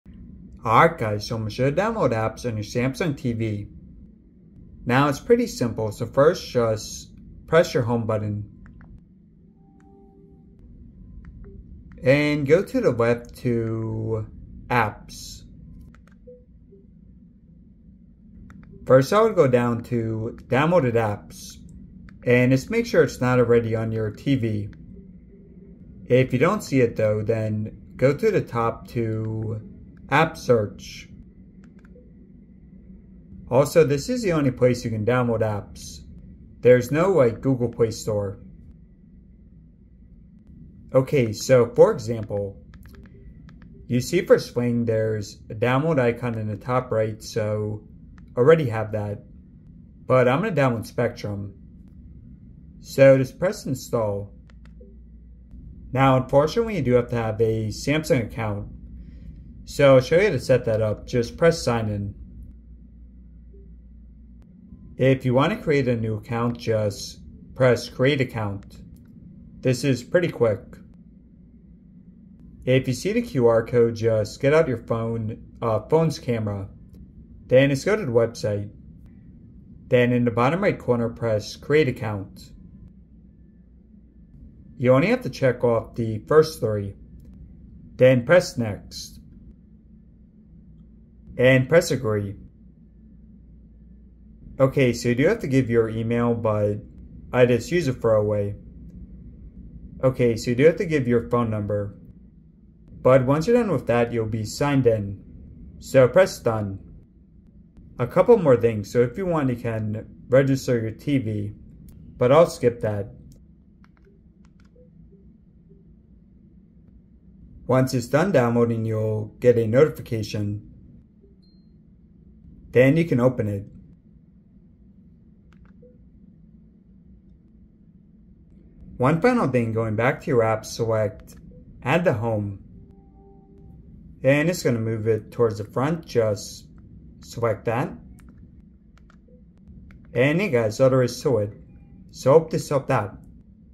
How to Download Apps on Samsung Smart TV!
How to download apps on Samsung Smart TVs - no Google Play Store is available. UHD 4K LED LCD AU8000 used but also works on any modern Samsung Smart TV.